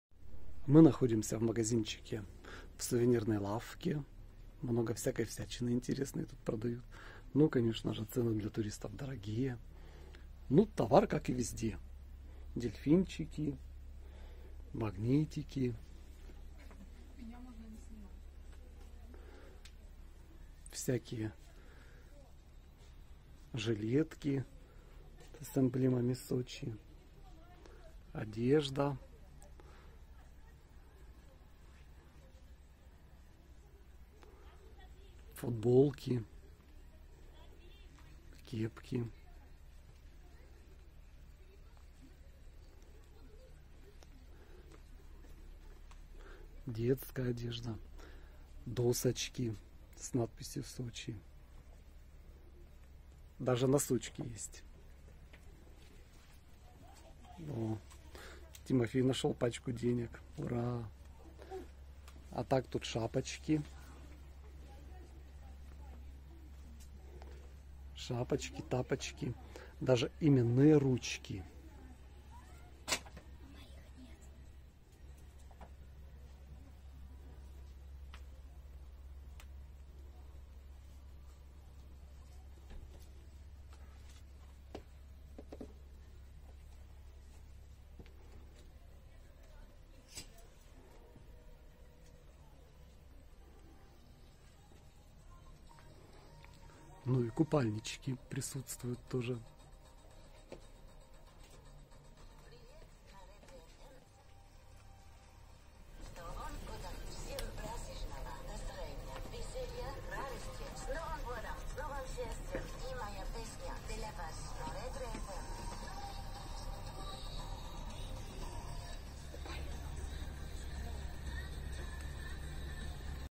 Пансионат Бургас в Сочи. Что продают в магазинчиках рядом
Что продают в сувенирных магазинчиках на территории пансионата. Приехав в Сочи зимой мы не ожидали, что будет так тепло. Поэтому возникла необходимость прикупить вещей полегче. Ведь мы прилетели из холодной Москвы. Обзор товаров в одном из магазинов для туристов на территории отеля